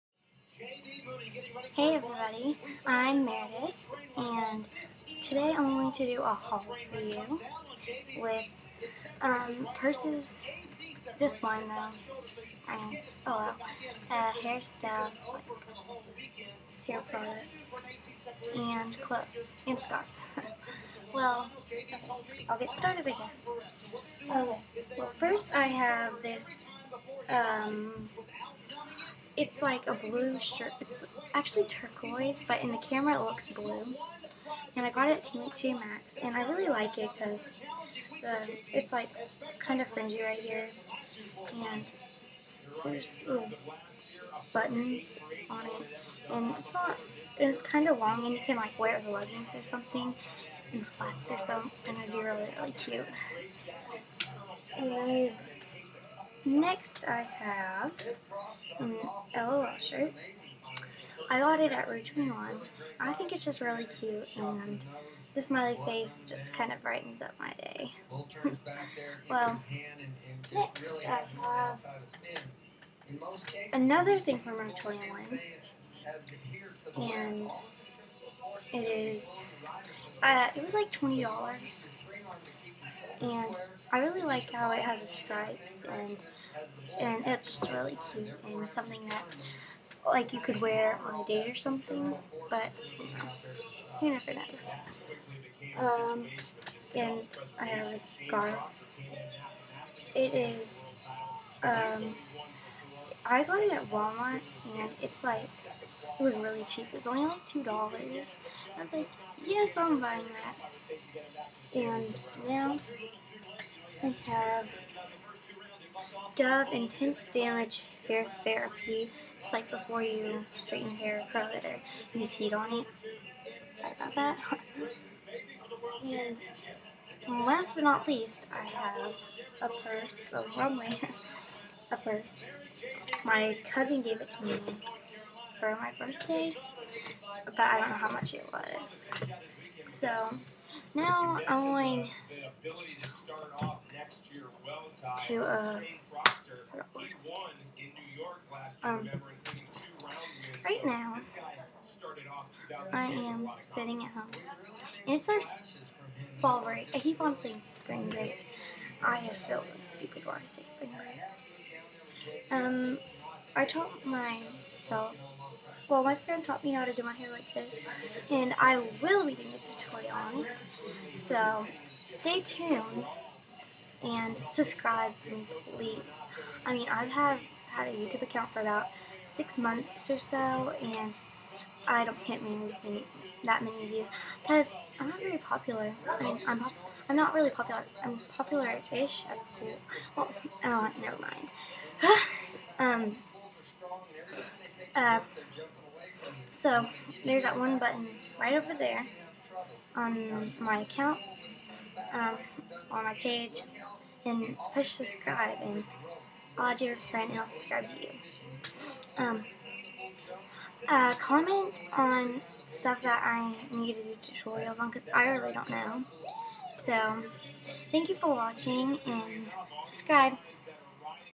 clothing and hair haul
me doing a haul with a leather perse, dove heat shield spray, scarf, rue 21 shirts and a shirt from t.j max well at least i think, p.s i have not made a video in a long time and i like mess up at times but i try so don't comment about that please